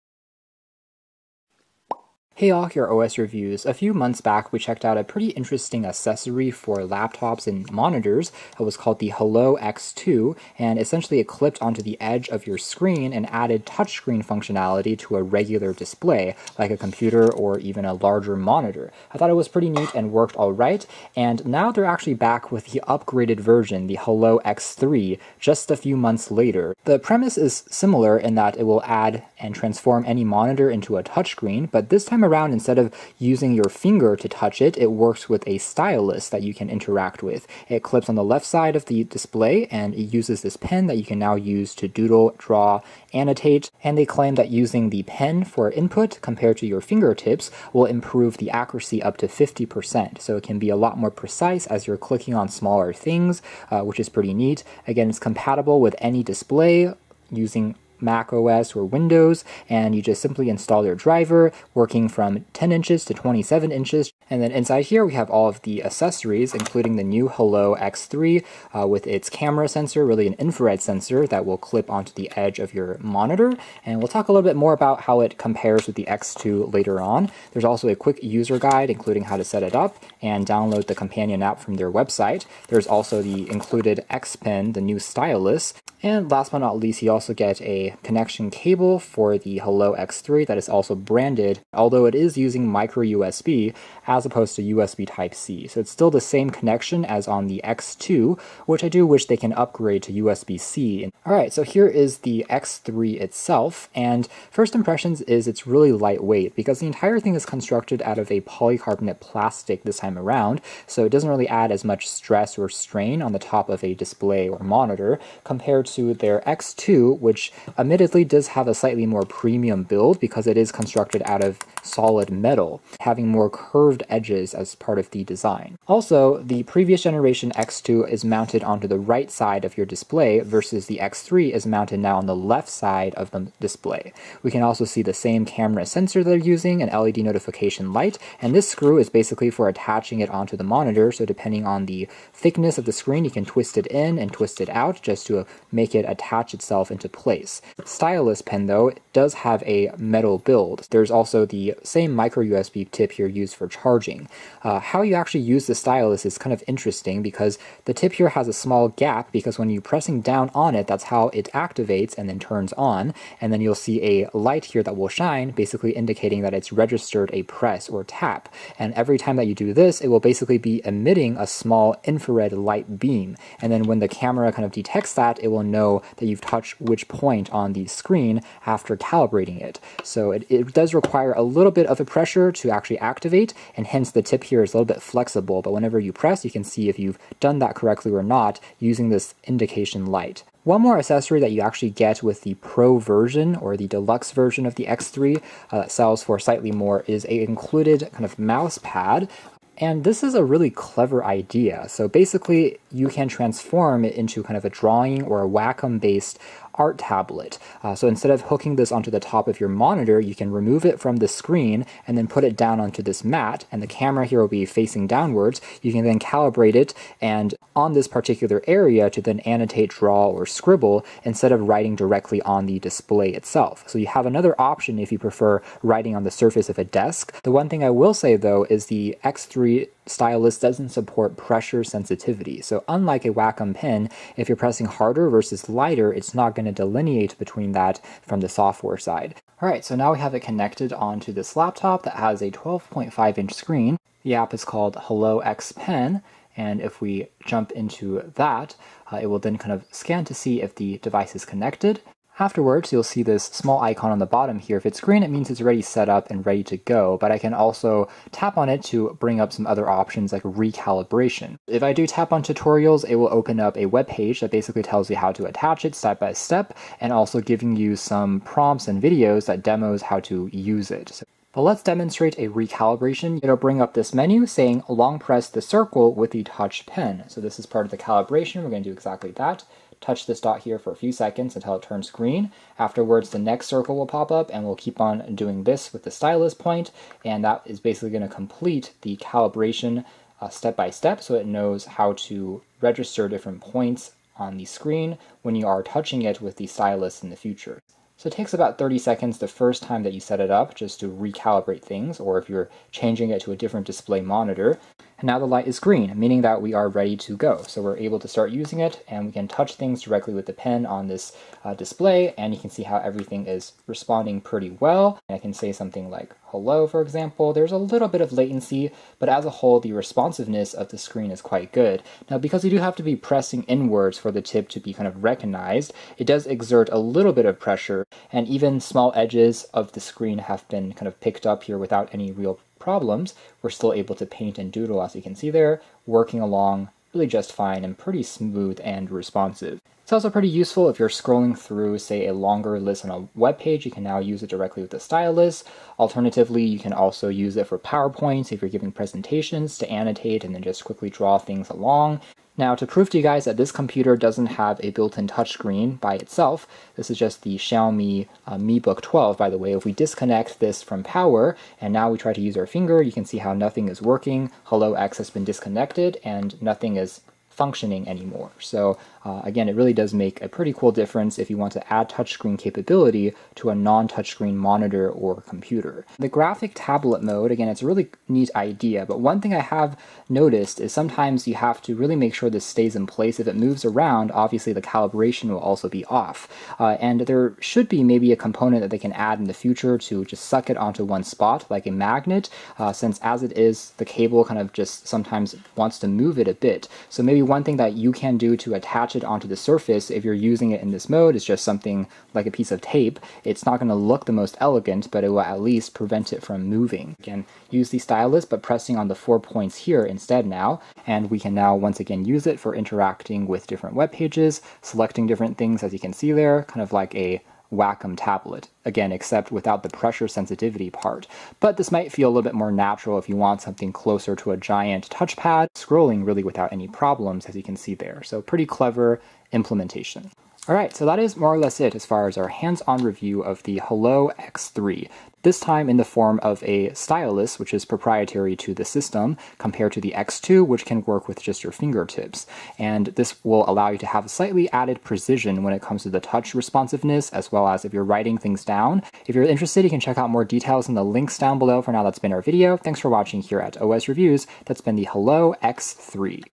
REVIEW: Hello X3 - Turn Any Monitor Into Touchscreen!? Updated Design w. Stylus Pen
YELANG Hello X3 peripheral - add touchscreen support to any laptop display

Details:
YELANG Hello X3 is an easy-to-mount and simple-to-use peripheral that is used on any standard desktop monitor or laptop to instantly transform it into an interactive touchscreen with extra functionality.

With the help of its built-in camera and infrared light bulbs, YELANG Hello X3 detects your moving finger towards the monitor and translates it into touch and drag mouse actions with high accuracy and no latency. It makes your work and fun more efficient and engaging.

1. Compared with the last generation, Hello X3 is more accurate in its touch, the total accuracy is greatly improved from 3mm to 2mm by 50%.
2. For Hello X3, you don't need to calibrate by adjusting the knob.
3.Objects around the screen will not interfere with the touch.
4.The touch effects are the same on all the screens for this generation. While for the Hello X2, the touch effects are better when there are no differences between the screens and the frames.

High Precision In Writing
A notebook with a 16:9 display in the size 13.3”, 14” or 15.6” running Windows 10. You need 22 mm of the flat surface below the bottom edge of the display for mounting the Hello X3 sensor.

USB Plug-and-Touch
After you've mounted your sensor correctly, just plug in the USB connector. If you need a longer USB cord, use an extension.
Intuitive Software Suite

Hello X3 makes it easy to move around in your presentations, it's a breeze to use for editing and really brings you a seamlessly with work or entertainment.

New Stylus (X Pen) design
Hello X3 supports all native gestures in Windows 10. Swipe away!

Turn your laptop into a tablet!
The next generation clip-on optical movement sensing technology that unlocks the virtual touching feature of your devices

Easy Installation AND Set Up
Hello X3 is easy to mount on monitors of all sizes from 10” to 27”.Simply clip it onto the corner of your screen. Supports Windows & Mac OS

The  Hello X3 doesn’t require a bare finger to detect touch. Gloves, paintbrushes and stylus will all work.

TURN ANY MONITOR INTO A TOUCH SCREEN
Hello X3 turns your computer into a giant touchscreen tablet. Unlike any other computer set boxes, the Hello X3 makes your computer truly smart and interactive.

Make Any Screen A Touchscreen.
Drawing on the computer, fine positioning, the error is less than 2mm,
Put Hello X3 on the desktop and turn it into a tablet.
Make your business presentation and signature more efficiently.
Hello X3, make a touch screen anytime, anywhere.

Includes X-Board accessory
Transforms into graphic art tablet mode (desktop mode)
Draw & doodle on a flat surface like Wacom tablet, or in regular touchscreen mode.